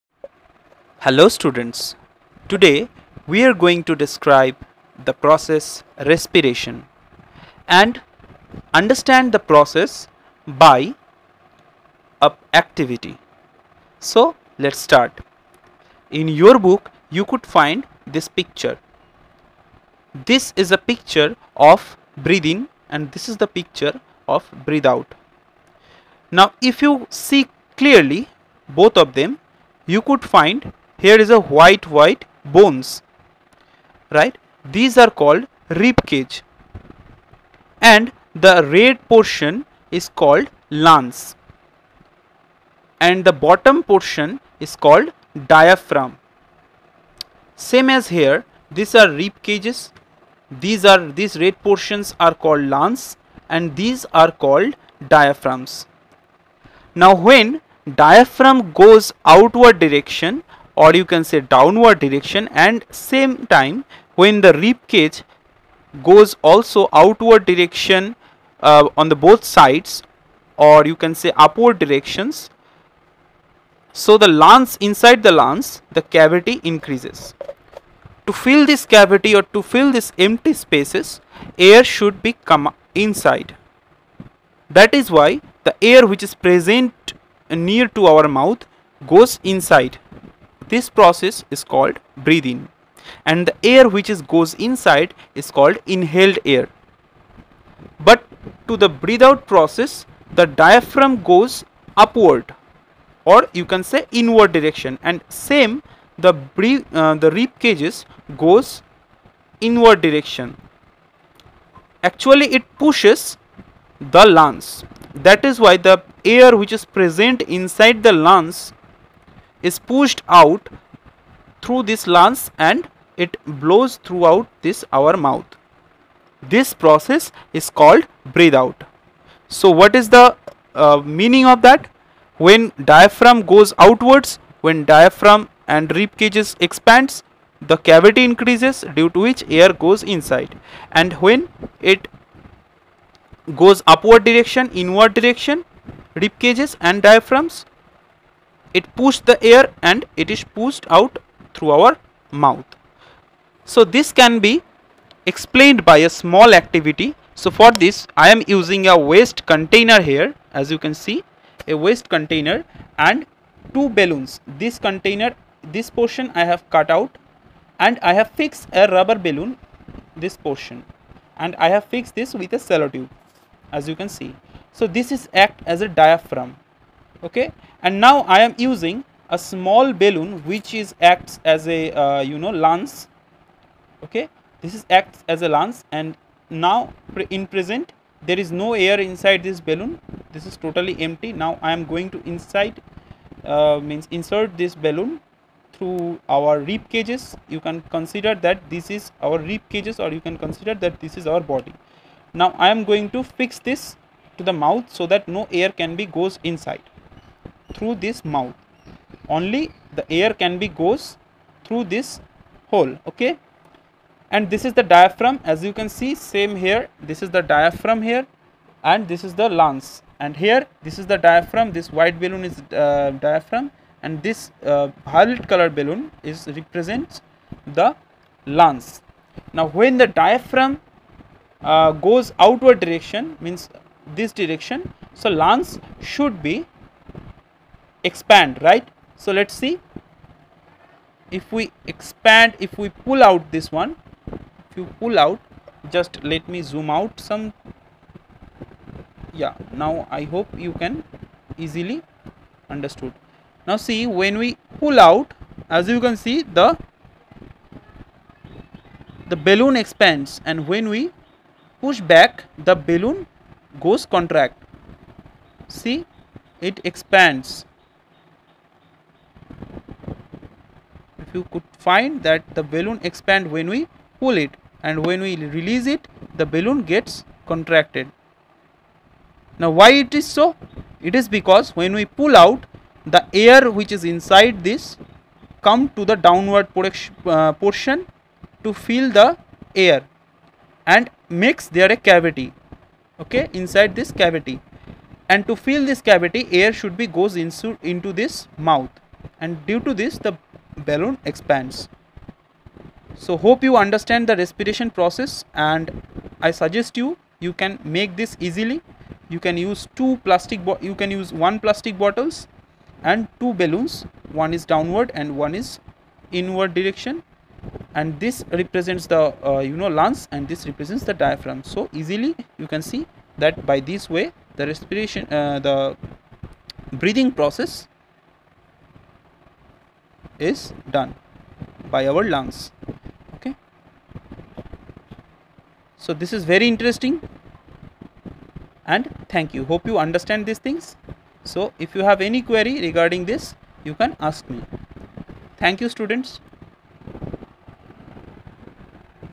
RESPIRATION ACTIVITY Demonstration
Hello students
In this video I have given a demonstration that how lungs work to breath in and out process

We need
1) wastes plastic bottle transperant
2) two rubber balloons
3) tape (to fix ballon)

The process is very easy.
I hope after seeing this video you can definitely make this by yourself.

For any query , ask in the comment Box